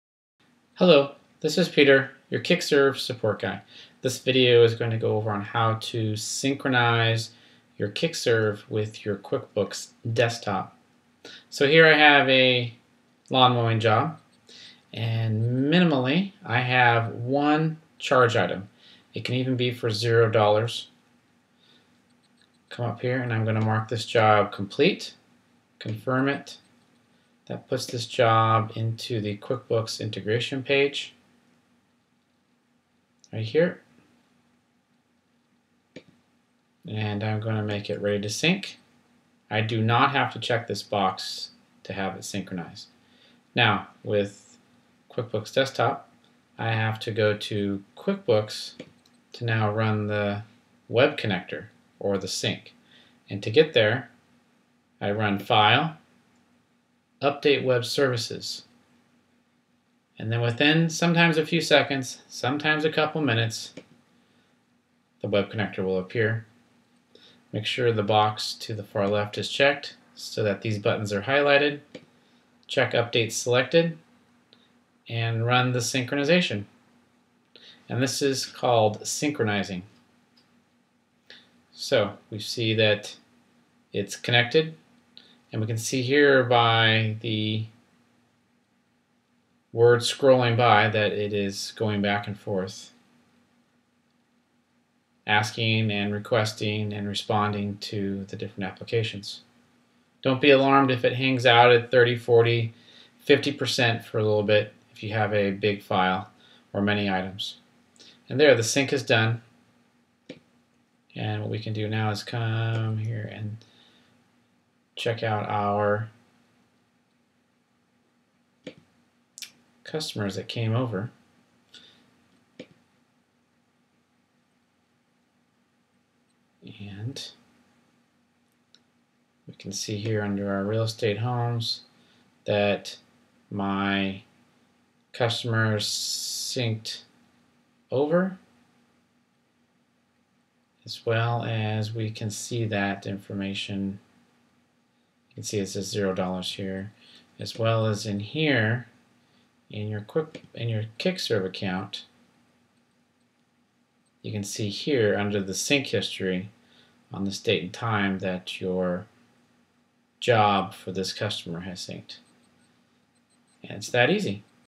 Kickserv - How to sync to Quickbooks
This video will demonstrate how to sync your Kickserv job so that it syncs to Quickbooks.